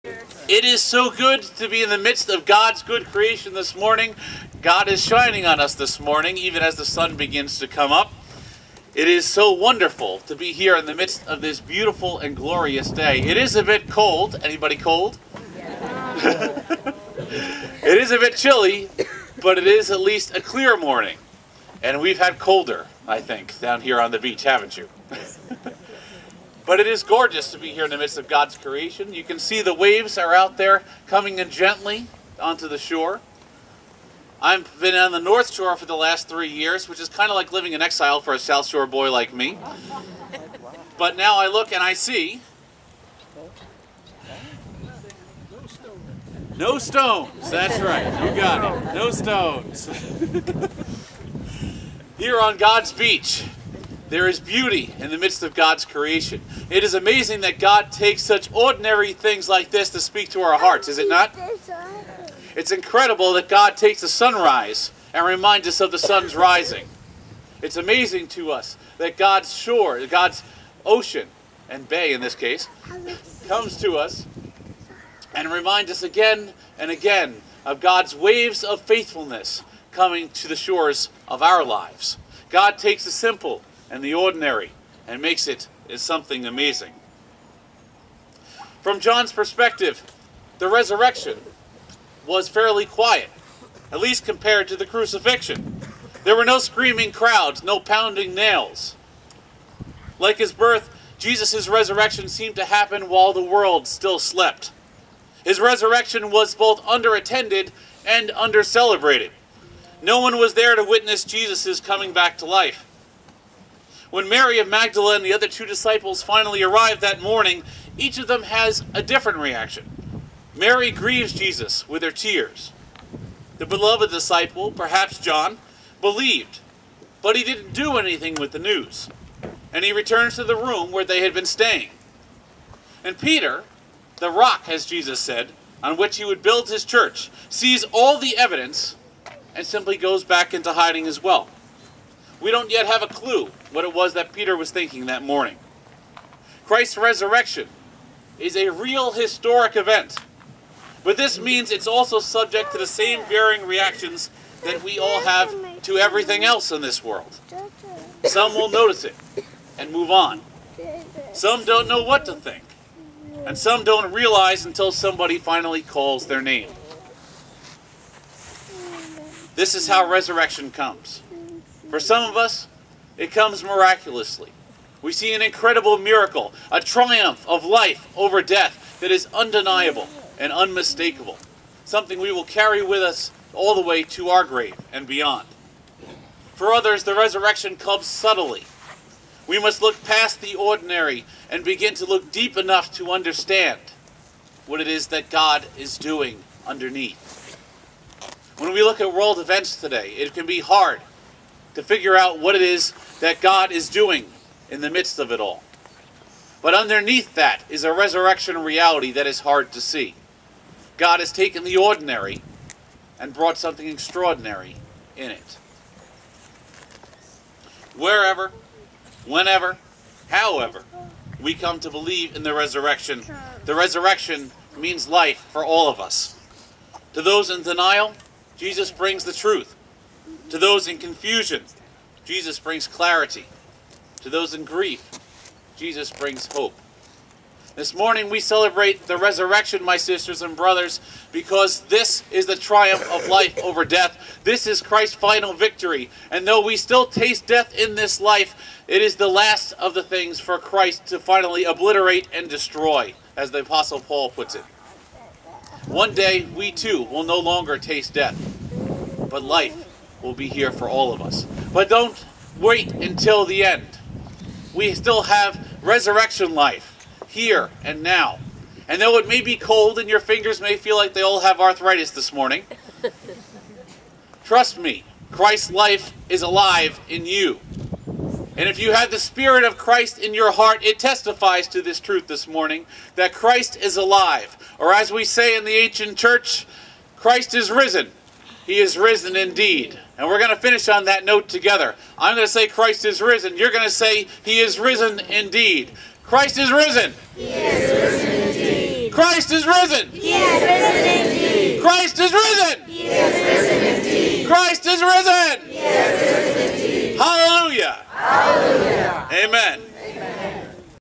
BUMC Sermon 3-27-2016 Easter Sunrise
Sermon:  "A Quiet Renewal"
By Rev. David Czeisel

A Celebration of Easter at Corey Beach, Blue Point, NY